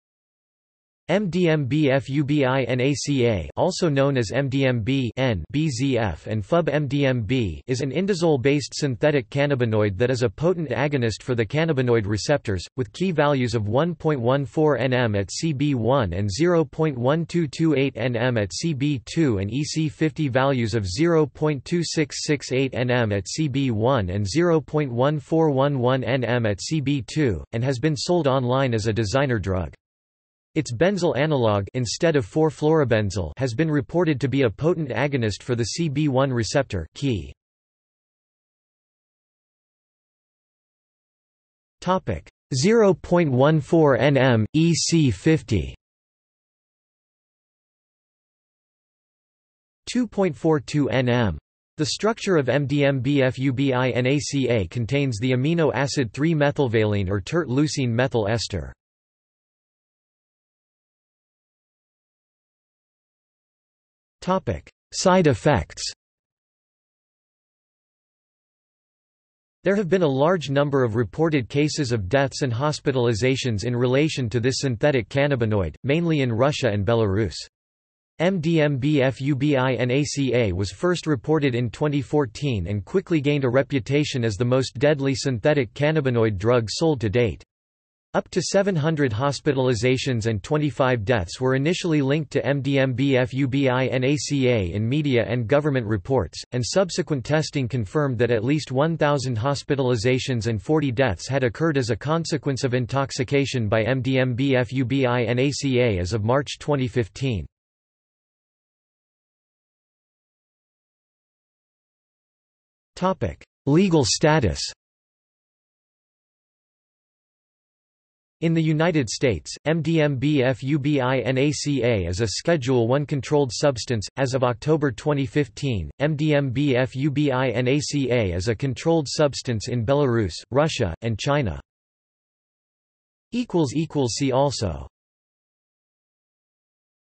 MDMB-FUBINACA | Wikipedia audio article
This is an audio version of the Wikipedia Article:
MDMB-FUBINACA

00:00:37 1 Side effects
00:01:00 2 Legal status
00:01:49 3 See also

- Socrates

SUMMARY
MDMB-FUBINACA (also known as  MDMB(N)-Bz-F and FUB-MDMB) is an indazole-based synthetic cannabinoid that is a potent agonist for the cannabinoid receptors, with Ki values of 1.14 nM at CB1 and 0.1228 nM at CB2 and EC50 values of 0.2668 nM at CB1 and 0.1411 nM at CB2, and has been sold online as a designer drug. Its benzyl analogue (instead of 4-fluorobenzyl) has been reported to be a potent agonist for the CB1 receptor (Ki = 0.14 nM, EC50 = 2.42 nM). The structure of MDMB-FUBINACA contains the amino acid 3-methylvaline or tert-leucine methyl ester.